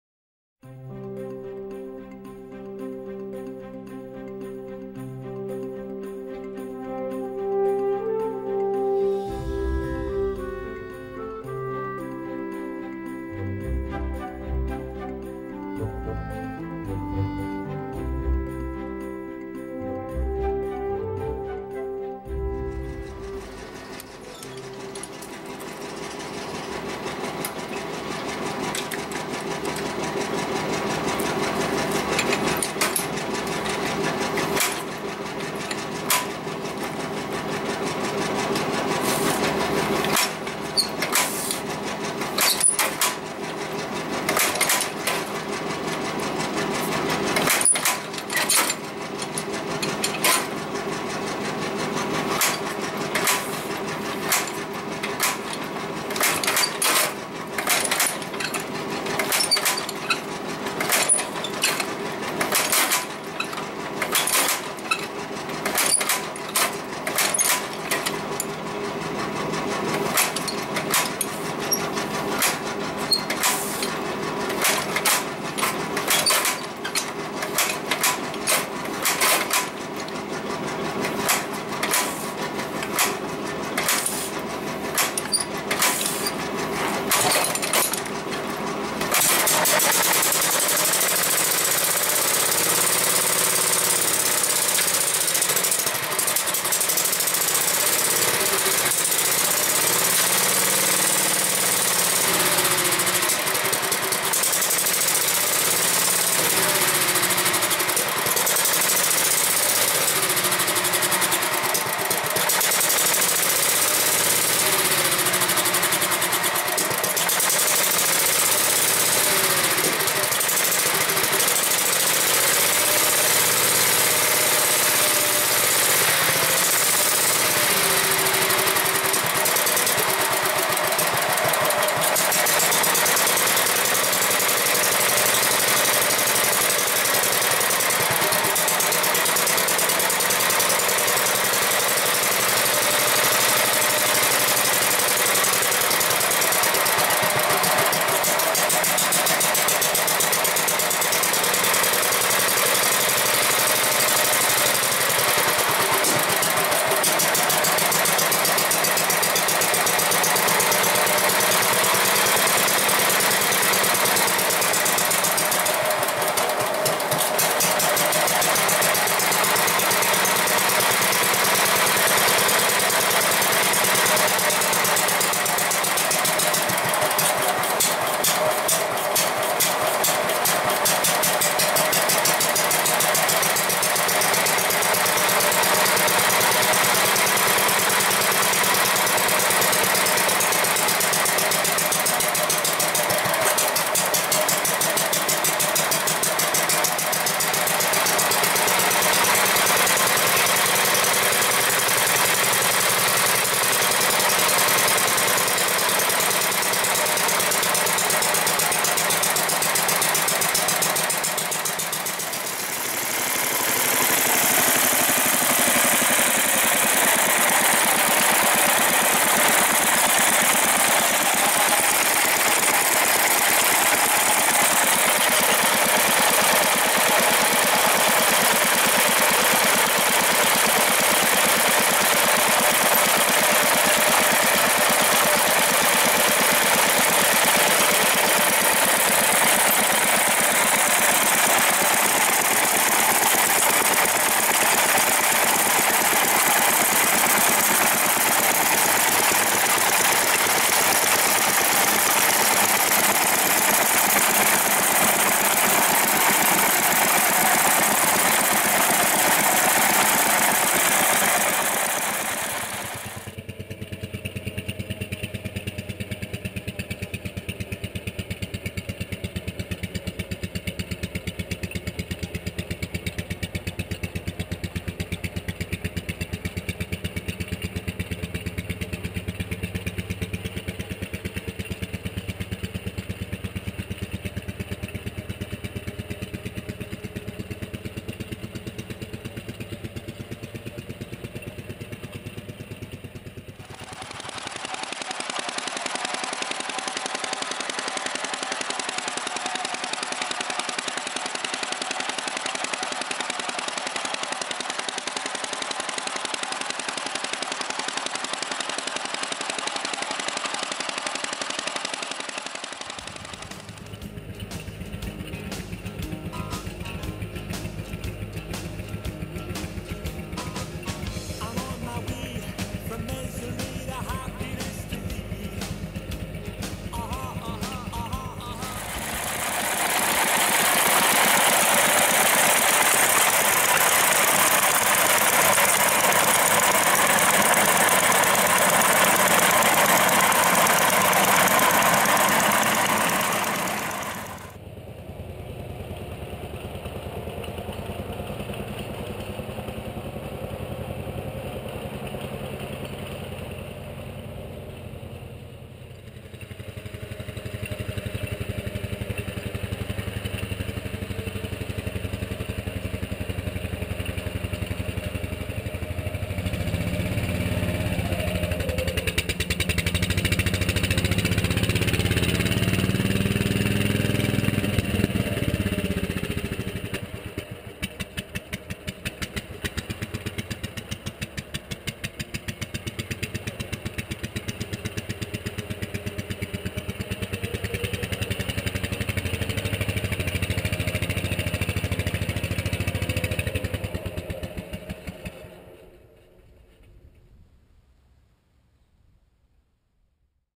Vaughan Mini Caterpillar
Starting up the Vaughan Flex-Tred for the first time in about seven months. Originally used for cleaning under chicken barns, this unique little tractor was built as a walk behind unit by the Vaughan Motor Company of Portland, Oregon. It was originally painted grey and had a small blade on the front. The engine is a license built 5hp Wisconsin and drives the machine through independent planetary clutches and a single speed transmission all immersed in an oil bath. Vaughan originally marketed these machines to small farms and orchards for use as a universal implement carrier and offered an entire catalogue of different attachments and implements  for various light duties. The advent of garden tractors all but sealed the fate of this unique design to the history books. Pioneer Acres Museum, Irricana, Alberta.